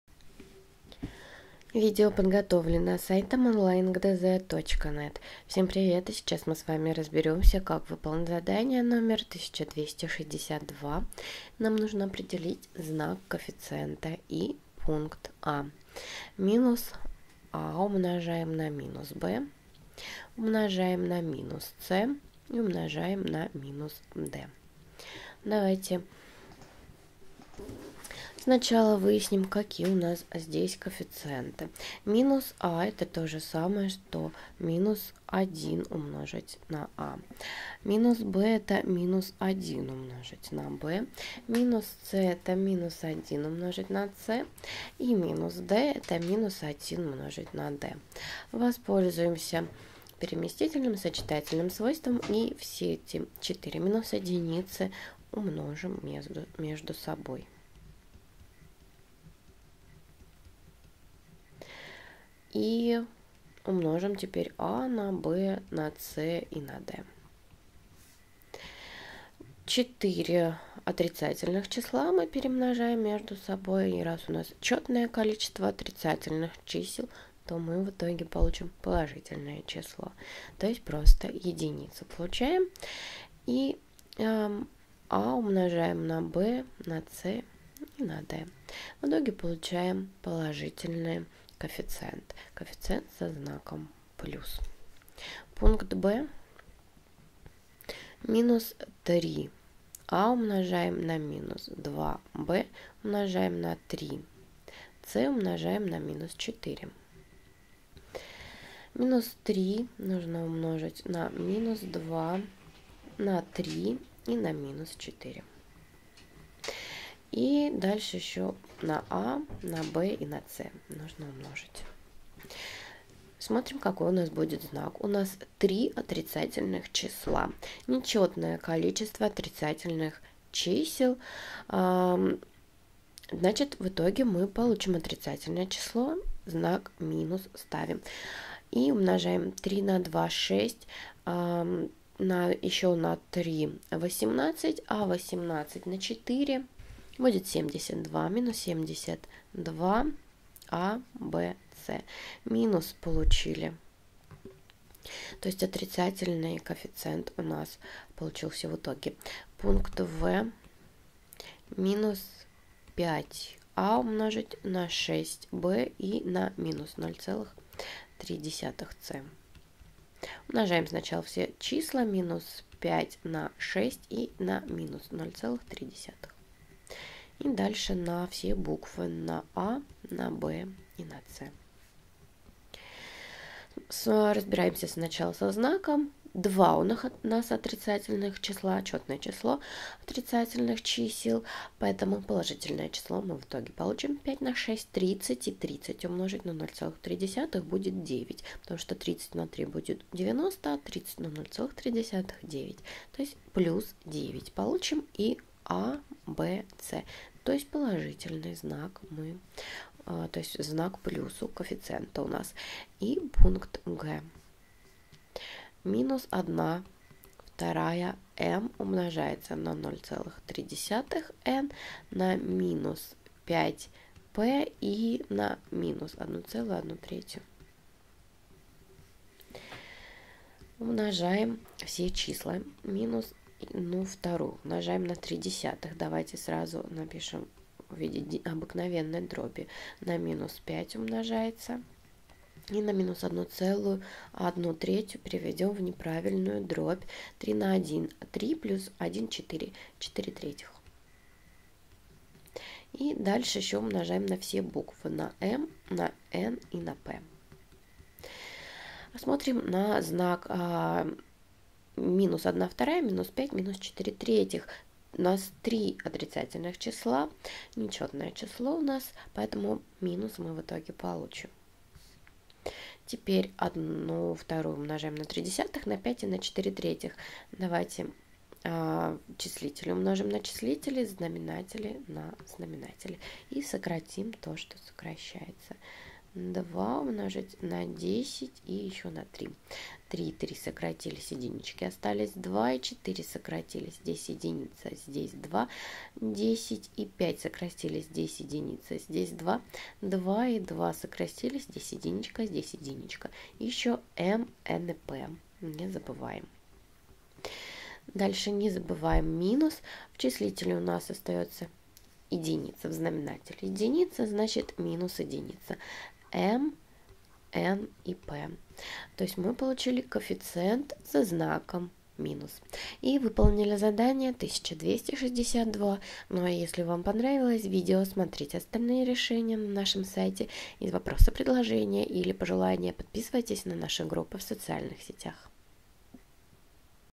Задание № 1262 - Математика 6 класс (Виленкин, Жохов)
В задании 1262 вам надо определить знак коэффициента(что такое коэффициент вы можете прочитать на странице 221). А для этого вы должны помнить свойства умножения рациональных чисел. Если вы хотите без ошибок сделать это задание, посмотрите наше видео.